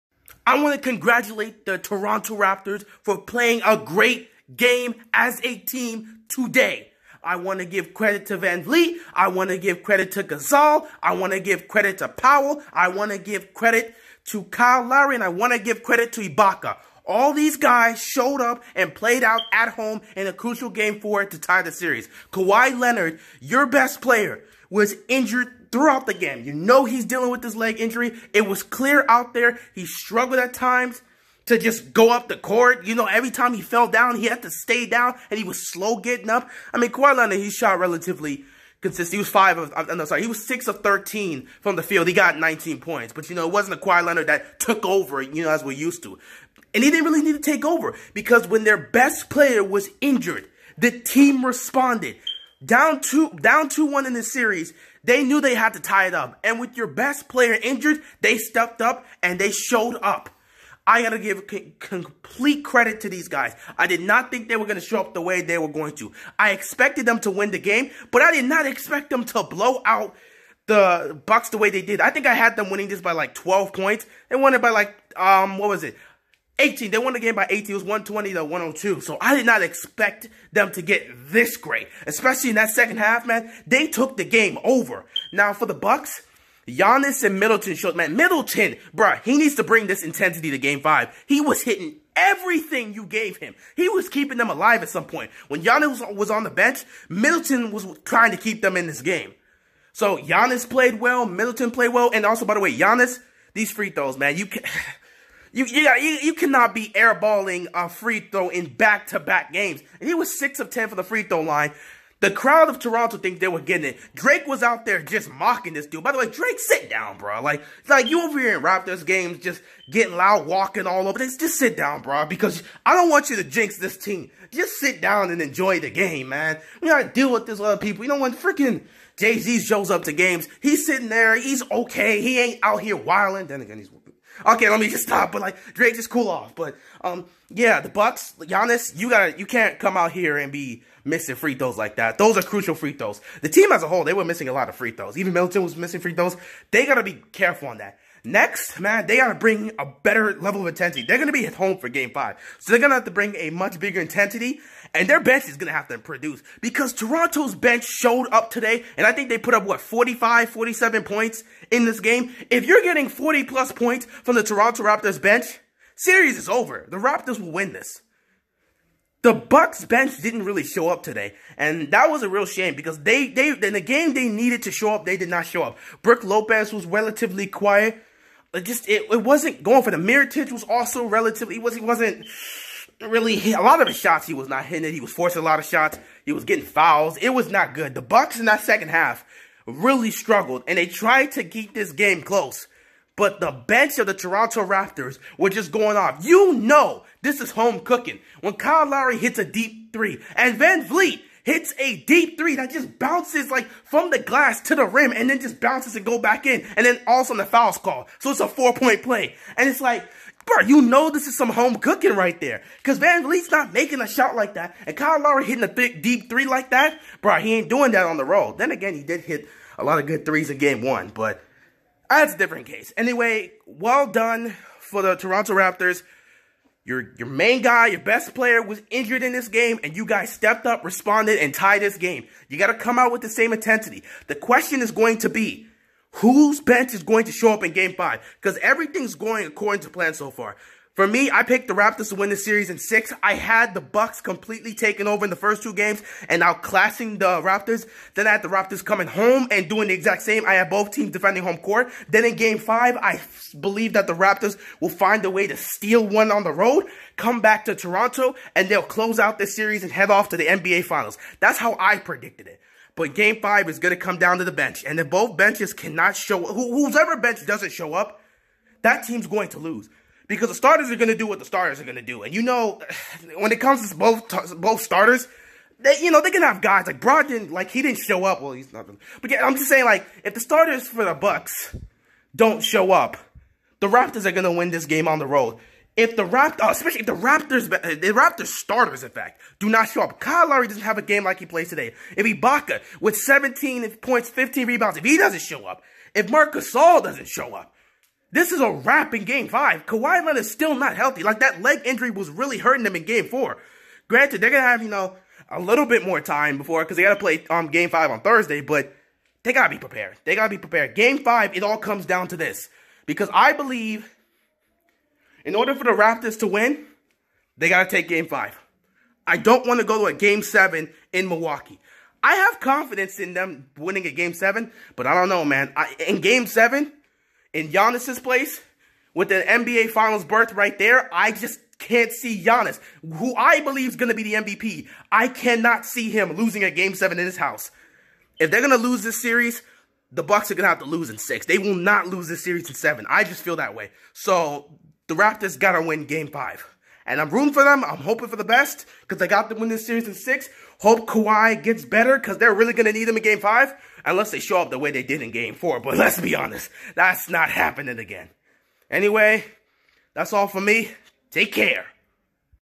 THE RAPTORS SHOW UP!!! Raptors even up the series 2-2
Gotta give credit to the starters and bench of Toronto. They showed up tonight. If you are under the age of 13, stay the hell away from my content!!